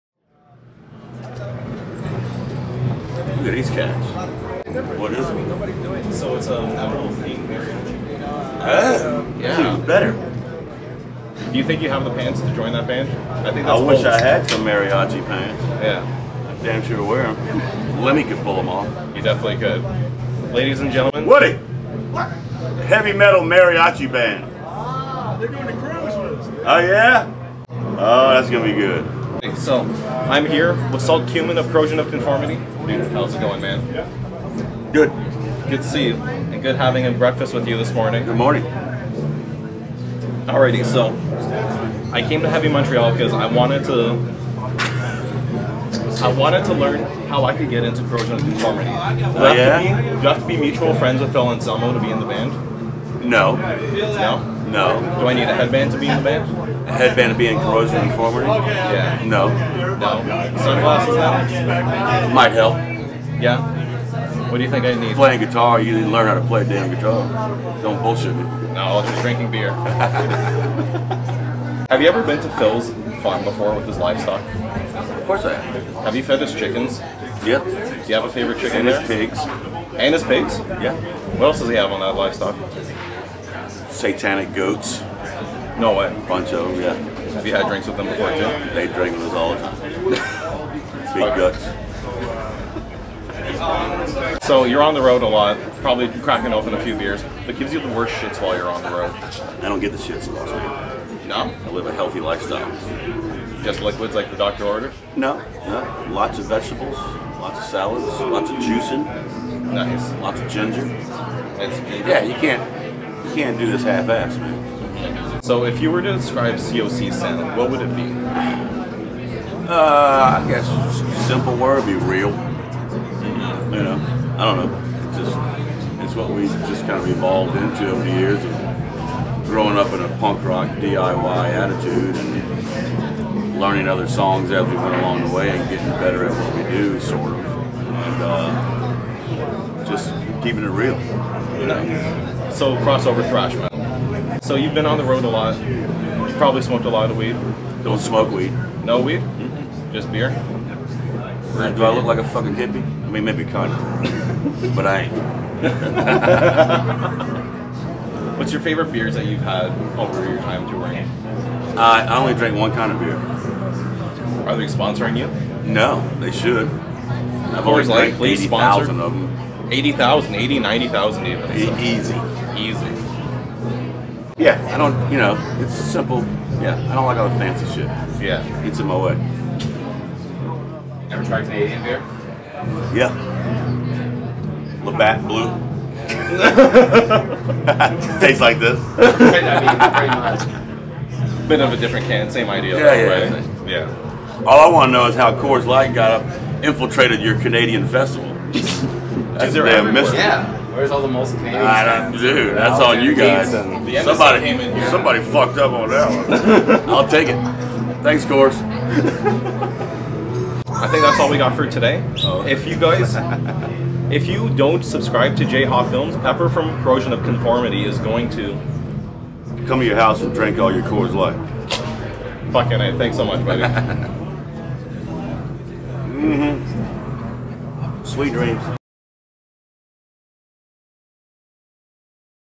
How to be PEPPER KEENAN
Chris interviews the legendary PEPPER KEENAN from Corrosion of Conformity and DOWN at Heavy Montreal 2019!

Follow us on shit!